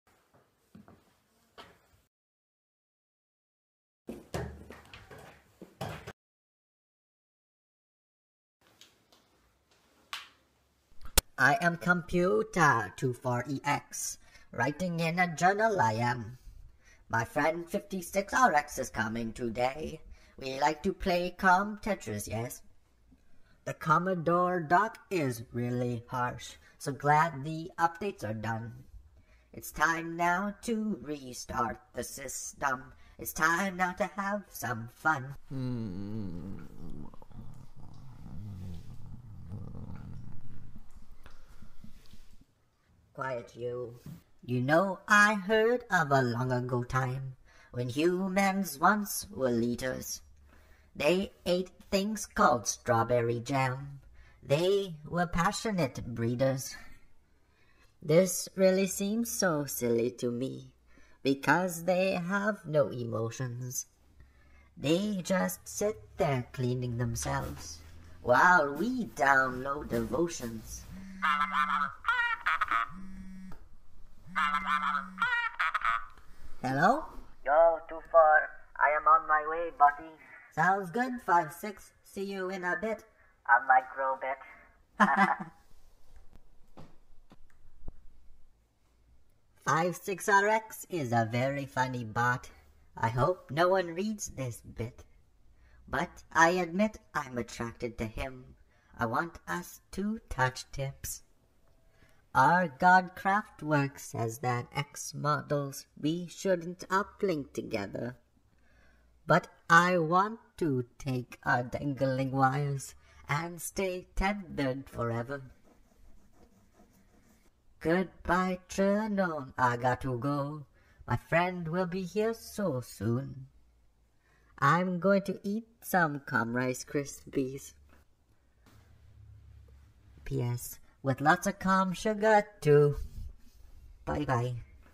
COMPUTER 24-EX VLOG JOURNAL #0001 - my friend is coming over!
hooray! I am computer happy as can be.  I am recording my thoughts,  and my feelings.  I can't believe there was a time when humans were the leaders! this is lo-fi.  My friend is coming today, and I may have feelings for him.